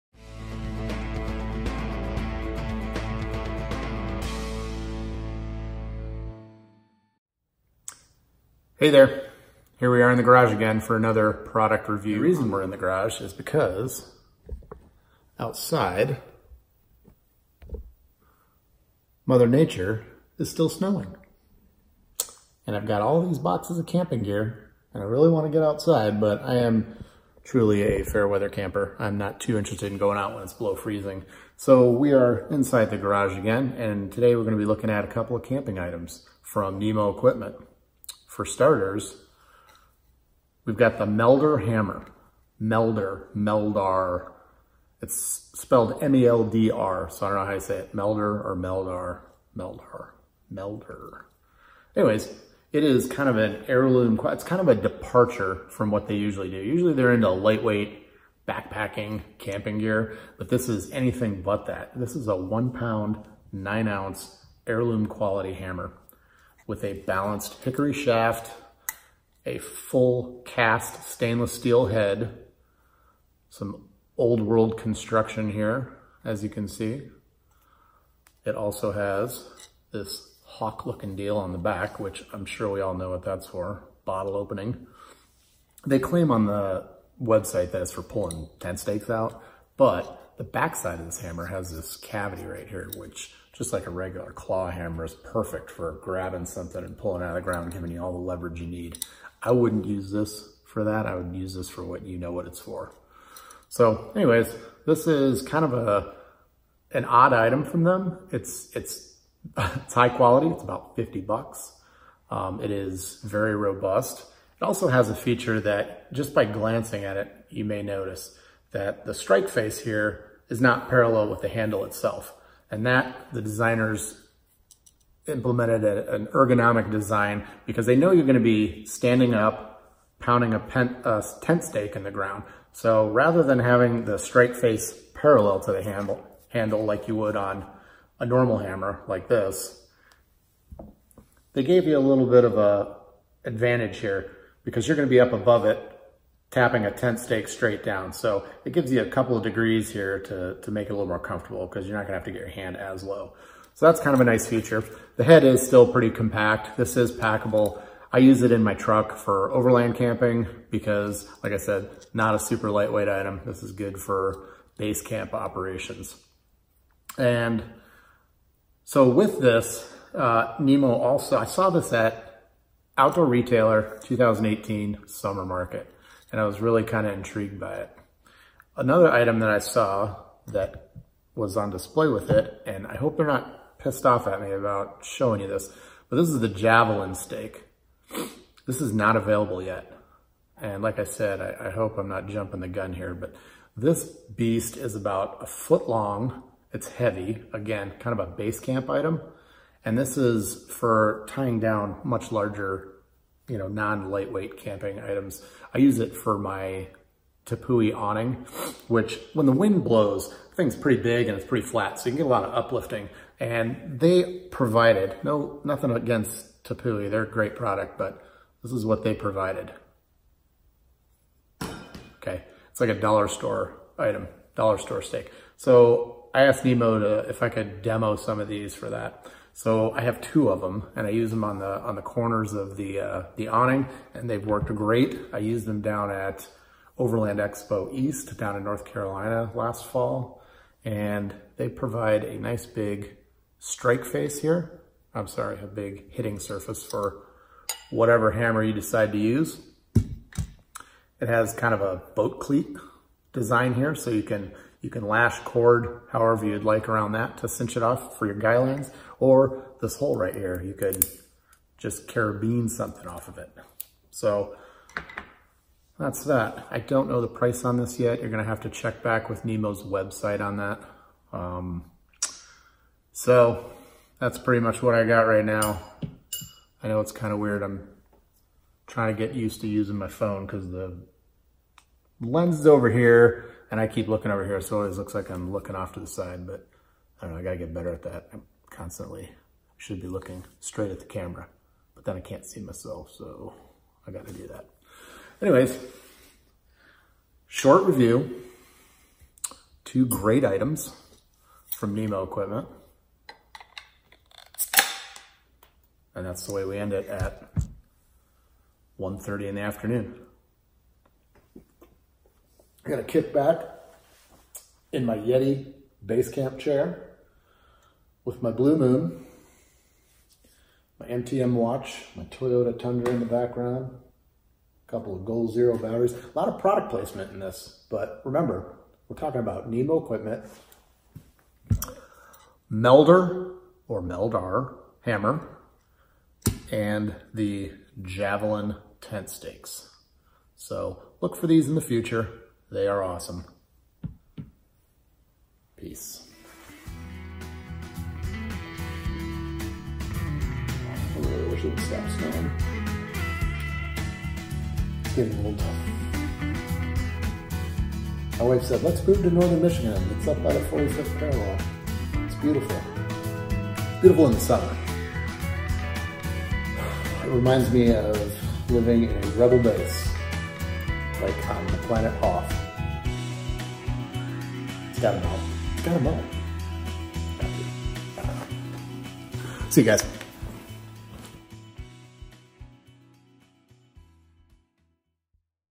Nemo Equipment Meldr Hammer Review
What I didn't expect, was to be drawn to an old school hickory shafted hammer. But for some reason, I was intrigued by it. As a General Contractor, I know my way around hand tools and more specifically, hammers.

Signing up to be notified on YouTube ensures you'll know immediately when new content is uploaded.